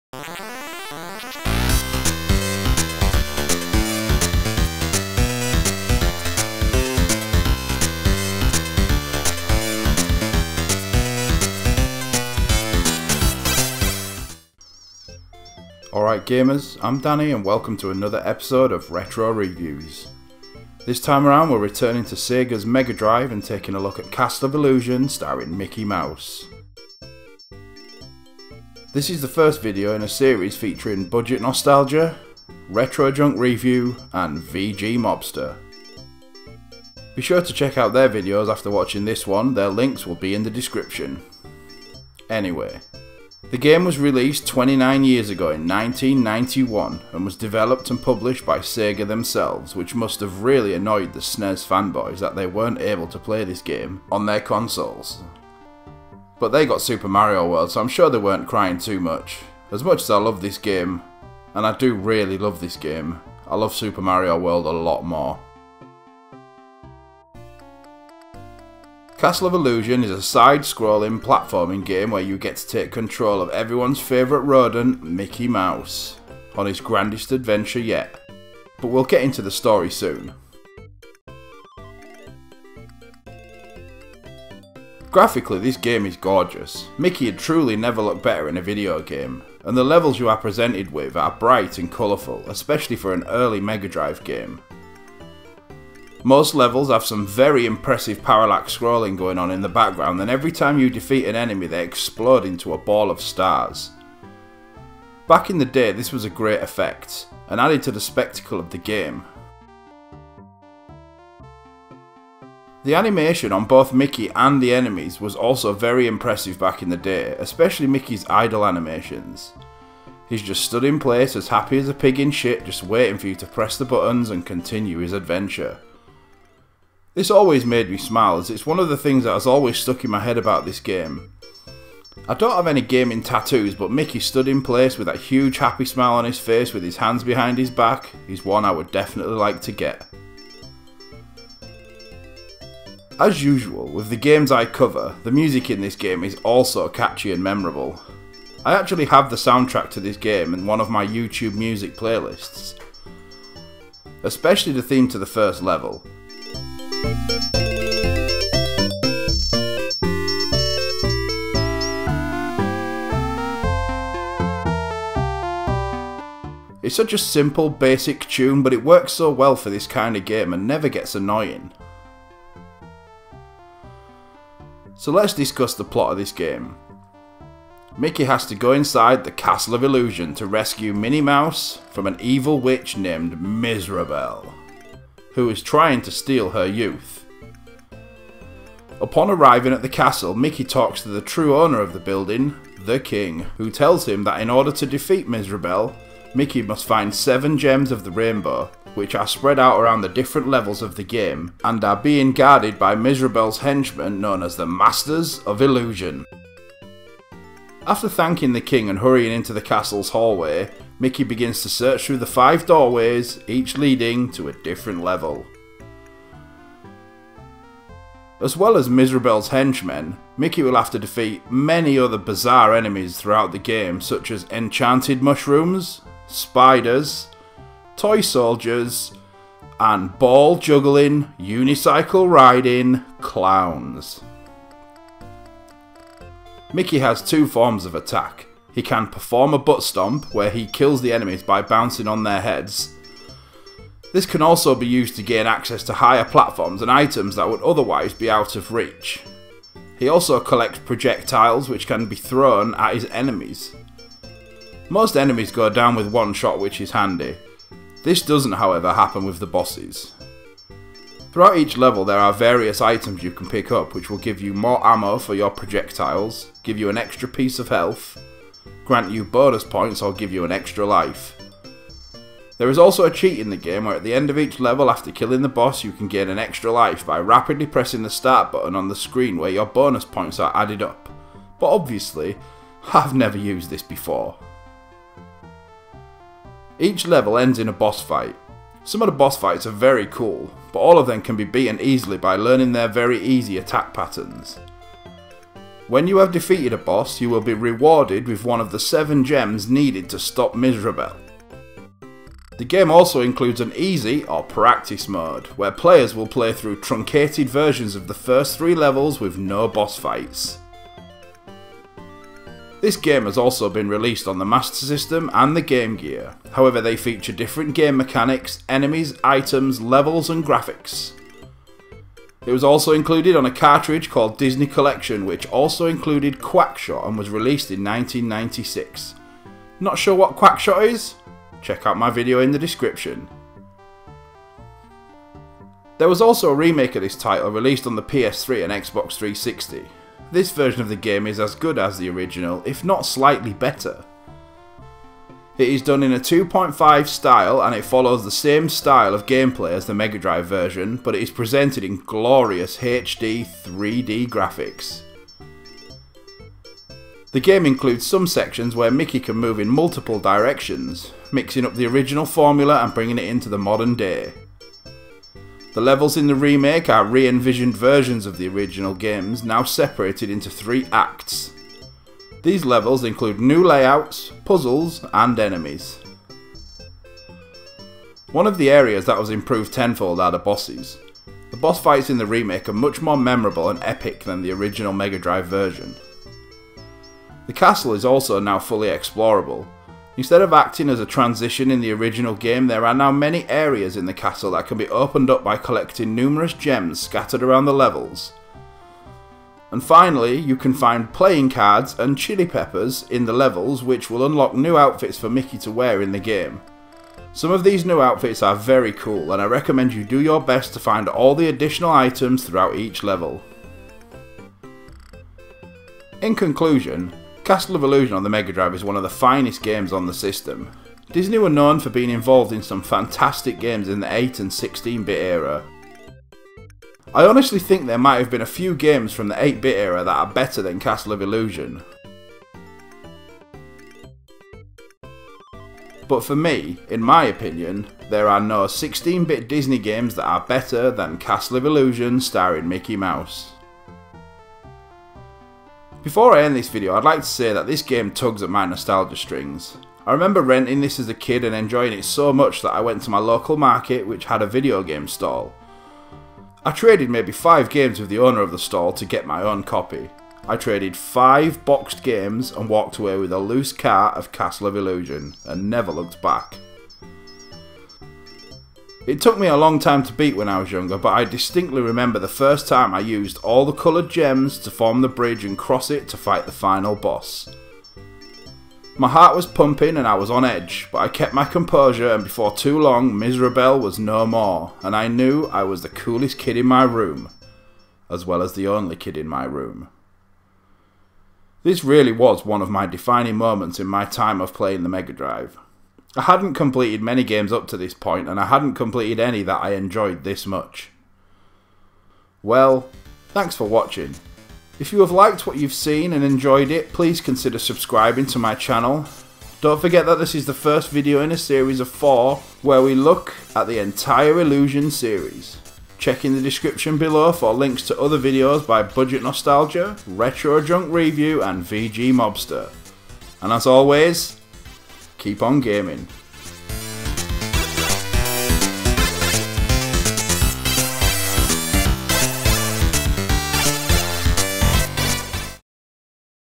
Castle Of Illusion - Mega Drive - Retro Reviews - Disneys best 16bit game?
Welcome to the next episode of Retro Reviews.

This episode is part of a four video series with Budget Nostalgia, Retro Junk and VG Mobster where we take a look at the complete illusion series staring Mickey Mouse and others.

Keep on gaming...